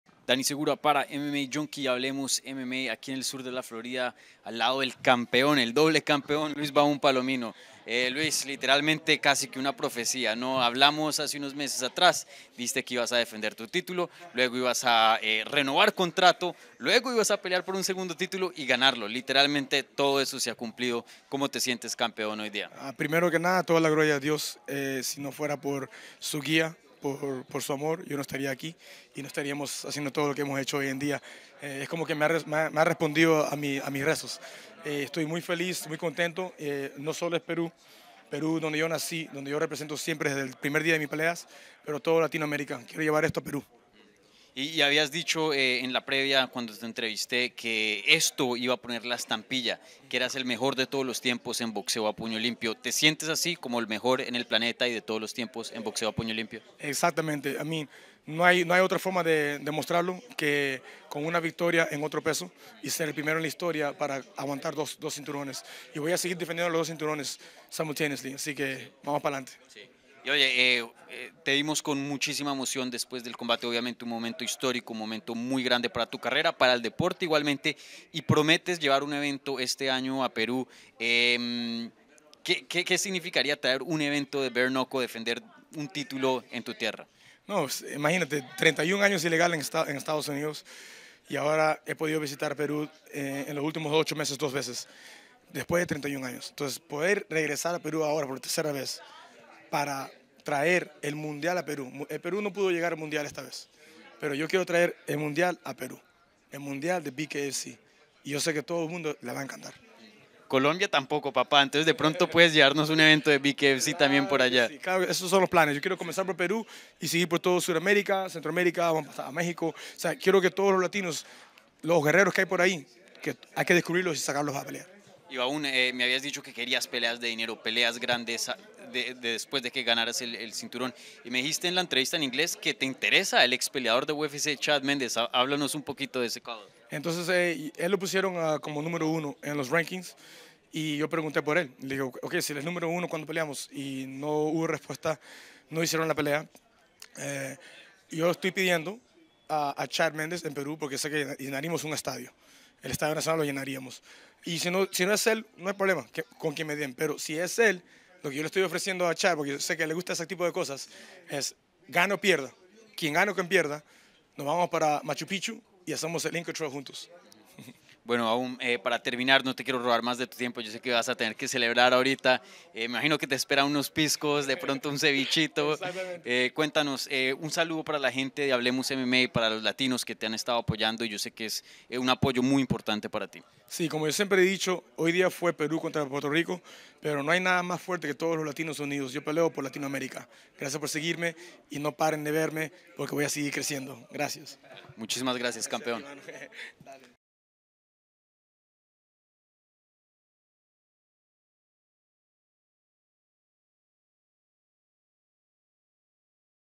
Luis Palomino pide a Chad Mendes: 'En Perú llenaríamos un estadio' | BKFC 26
Luis Palomino habla acerca de su victoria histórica en BKFC 26 ganando dos cinturones en dos categorías, deseos de pelear con Chad Mendes, y mucho más.

Sigue a HablemosMMA​